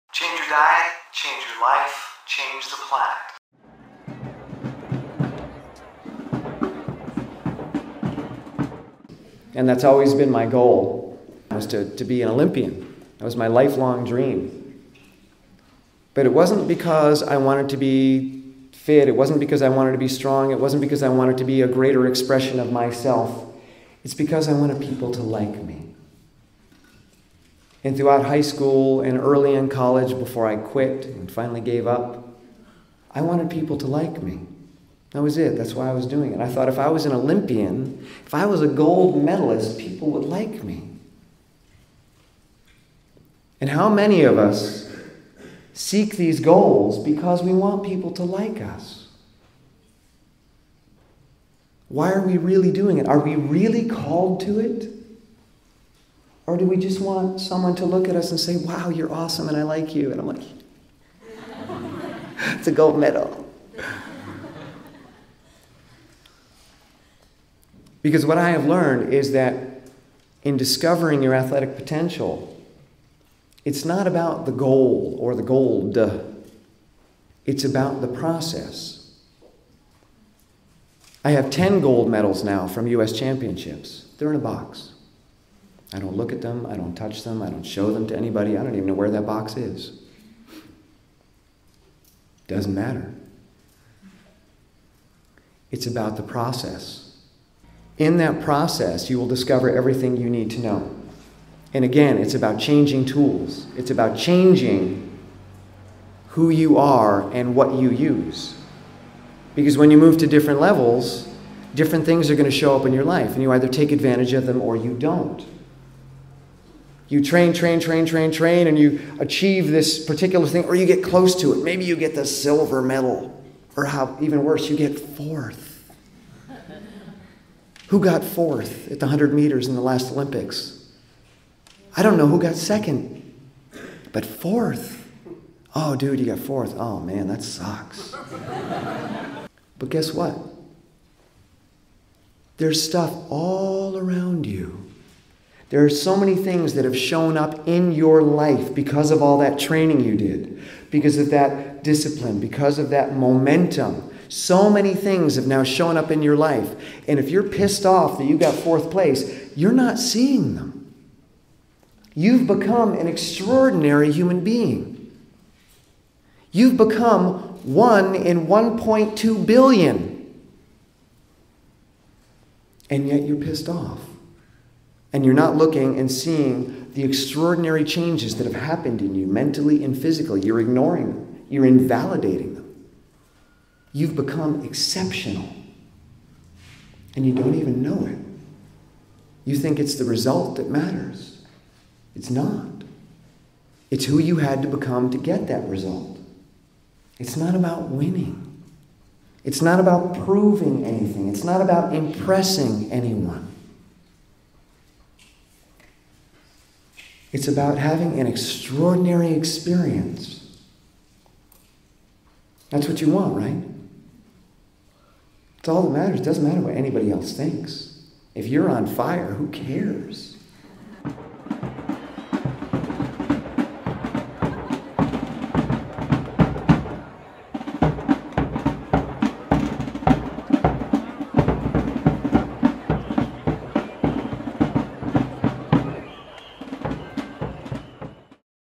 The Downside of Focusing on Your Goals
How to lose weight, get fit, succeed on a diet, deal with obstacles and change your life.
Do you have athletic goals? Why do you have them? What needs will they meet? Does athletic accomplishment make us happy? Does winning a competition lead to fulfillment? What drives us to do what we do as athletes?
This is a clip from my talk "Discovering Your Athletic Potential" at the 2013 Woodstock Fruit Festival.